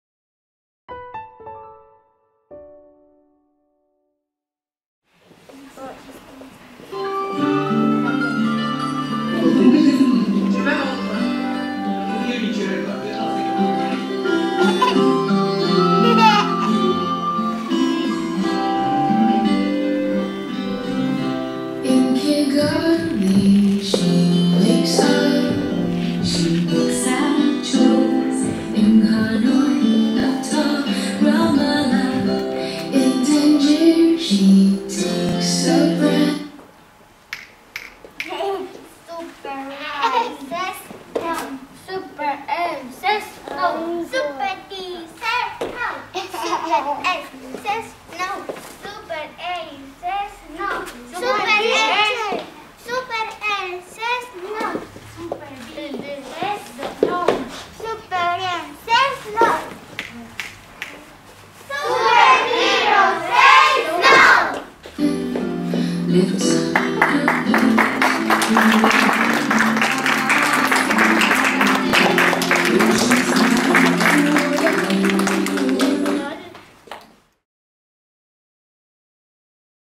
Superheroes Say No!!
Actividade realizada polo alumnado da sección bilingüe na conmemoración do Día contra a violencia de xénero